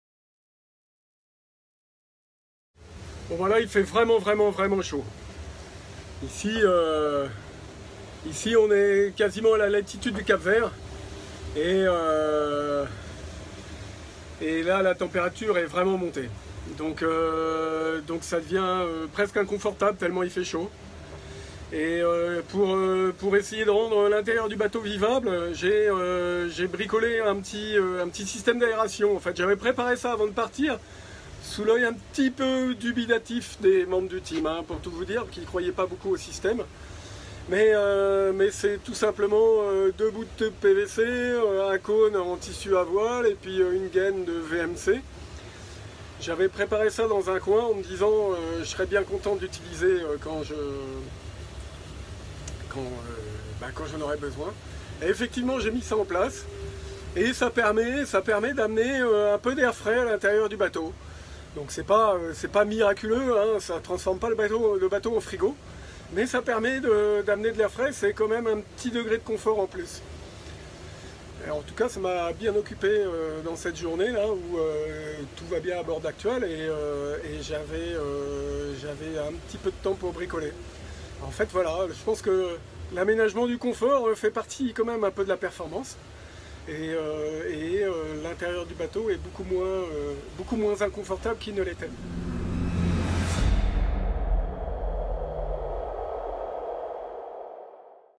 Aménagement à bord de l'Ultim Actual - Jour 6
Bricolage à bord d'Actual Ultim. Yves Le Blevec procède à un petit aménagement à bord du trimaran géant : "L'aménagement du confort fait partie de la performance".